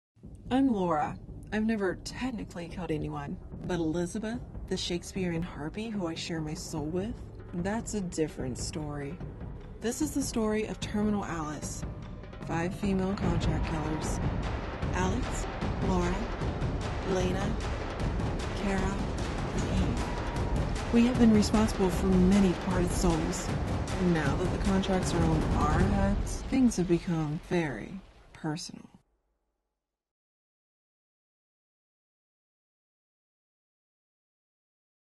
Terminal Alice Teaser Trailer
Trailer for the upcoming graphic novel TERMINAL ALICE. Produced by Big City Comics Studio, published by Zenescope. Debuting August 2011!